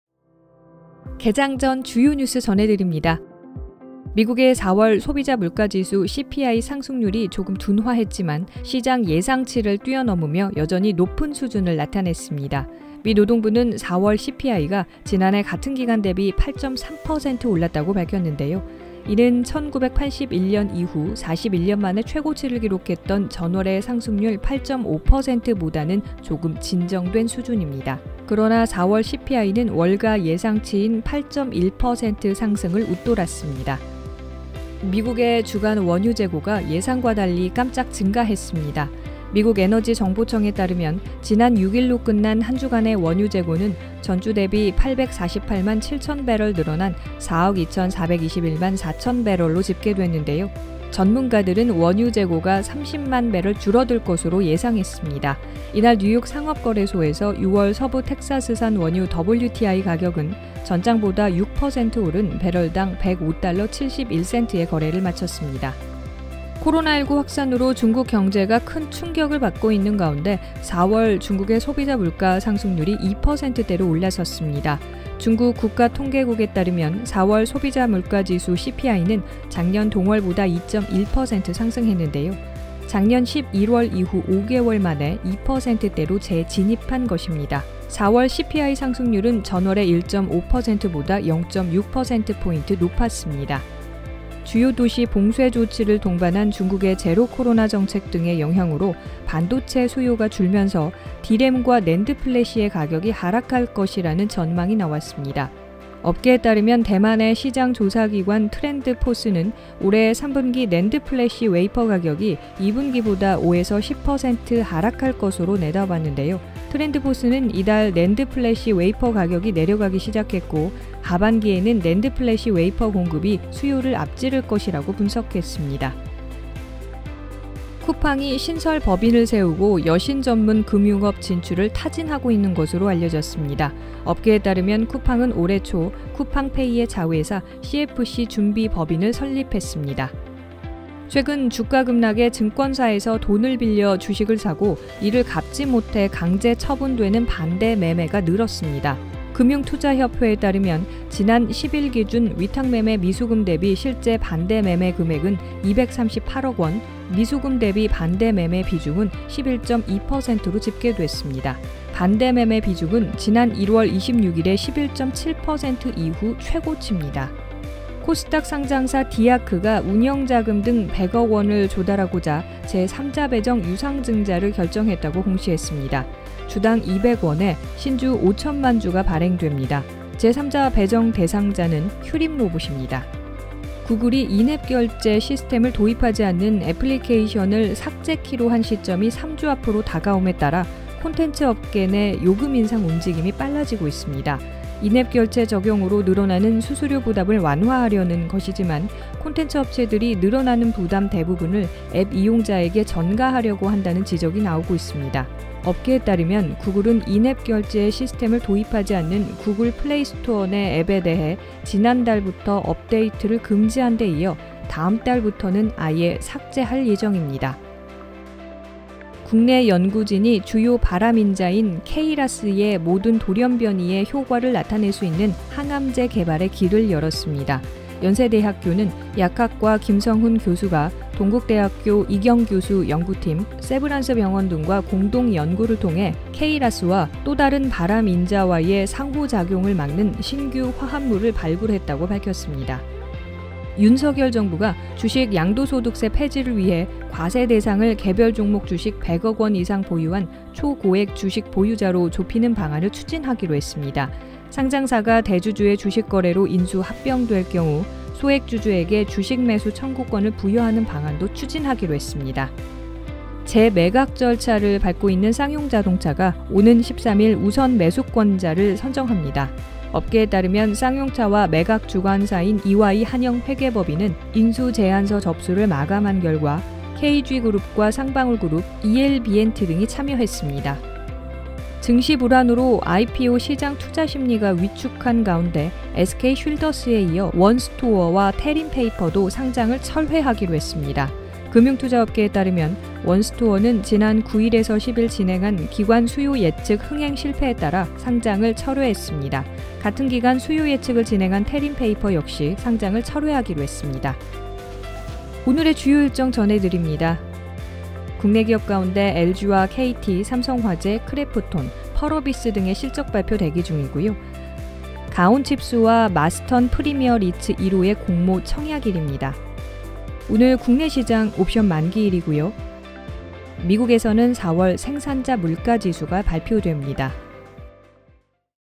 [0512개장] 개장전 주요이슈&일정
■주요 뉴스

●美 4월 CPI 전년比 8.3%↑…8개월만 둔화에도 예상치 상회
●EIA 주간 원유재고 848만7천배럴 깜짝 증가…WTI 상승세 유지
●中 물가 5개월만에 2%대로 올라…코로나·원자재값 상승
●중국 '도시 봉쇄'에 수요 타격…반도체 시장에도 '먹구름'

●쿠팡, 여신전문금융업 진출하나…금융 컨설팅 신설법인 설립
●코스피 2,600 붕괴, 반대매매도 증가…미수금 대비 비중 11%
●디아크, 100억원 유상증자...휴림로봇에 제3자배정
●구글 3주후 인앱결제 미적용 앱 삭제…콘텐츠업계 요금인상 확산

●국내 연구진, 암 인자 'KRAS' 결합 억제 방법 발견
●100억원 미만 주식보유자 양도세 폐지 추진…공매도 규제강화
●쌍용차 인수전에 KG·쌍방울 참여…우선매수권자 13일 선정
●증시 불안에 상장 철회 잇달아..원스토어·태림페이퍼도 철회

■주요 일정

●LG/KT/삼성화재/크래프톤/펄어비스 실적발표 예정
●가온칩스/마스턴프리미어리츠1호 공모청약
●옵션만기일
●美 4월 생산자물가지수